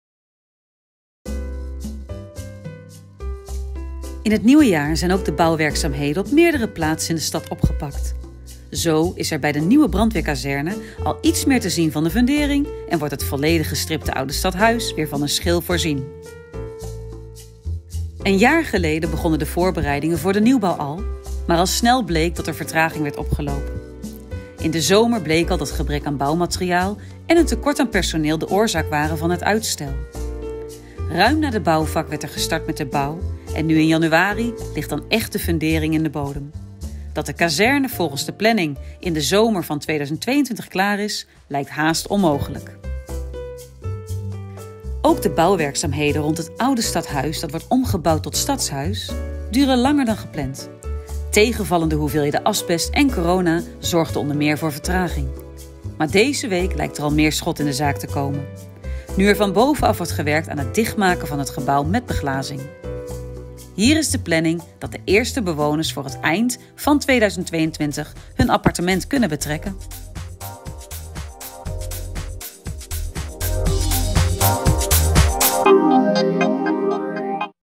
Bouwen in 2022 Stadhuys en brandweerkazerne
In het nieuwe jaar zijn ook de bouwwerkzaamheden op meerdere plaatsen in de stad opgepakt. Zo is er bij de nieuwe brandweerkazerne al iets meer te zien van de fundering en wordt het volledig gestripte oude stadhuis weer van een glasschil voorzien.

Een jaar geleden begonnen de voorbereidingen voor de nieuwbouw van de brandweerkazerne al, maar al snel bleek dat er vertraging werd opgelopen door gebrek aan bouwmateriaal en een tekort aan personeel. Dat gold ook voor het StadHuys dat nu eind dit jaar klaar moet zijn.